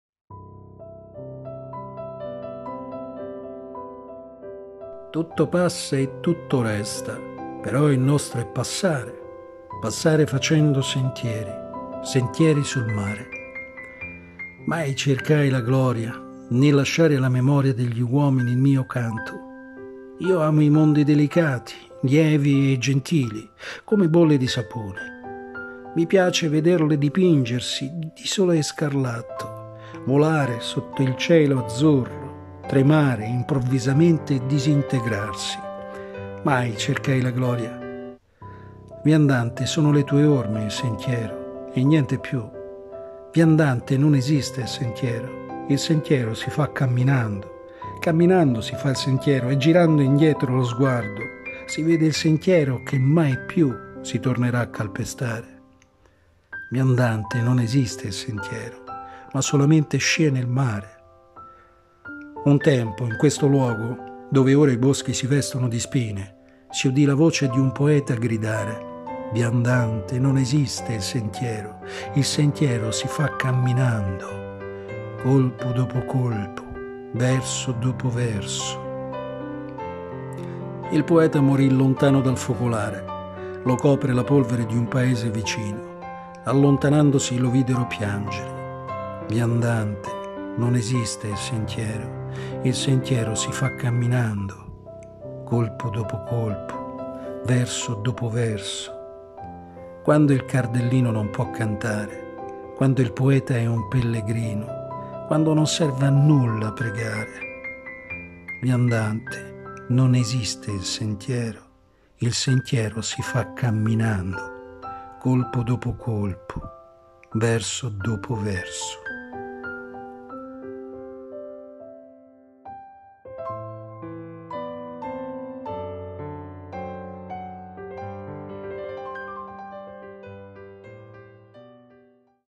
Caminante - Antonio Machado -
Antonio Machado

Caminante
(Voce Angelo Gullotti)

Tutto passa e tutto resta,
però il nostro è passare,
passare facendo sentieri,
sentieri sul mare.

Mai cercai la gloria,
né di lasciare alla memoria
degli uomini il mio canto,
io amo i mondi delicati,
lievi e gentili,
come bolle di sapone.

Mi piace vederle dipingersi
di sole e scarlatto, volare
sotto il cielo azzurro, tremare improvvisamente e disintegrarsi…

Viandante, sono le tue orme
il sentiero e niente più;
viandante, non esiste il sentiero,
il sentiero si fa camminando.

Camminando si fa il sentiero
e girando indietro lo sguardo
si vede il sentiero che mai più
si tornerà a calpestare.

ma solamente scie nel mare…

Un tempo in questo luogo
dove ora i boschi si vestono di spine,
si udì la voce di un poeta gridare «Viandante non esiste il sentiero,

Colpo dopo colpo, verso dopo verso…

Il poeta morì lontano dal focolare.
Lo copre la polvere di un paese vicino. Allontanandosi lo viderono piangere. «Viandante non esiste il sentiero,

Quando il cardellino non può cantare. Quando il poeta è un pellegrino,
quando non serve a nulla pregare. «Viandante non esiste il sentiero,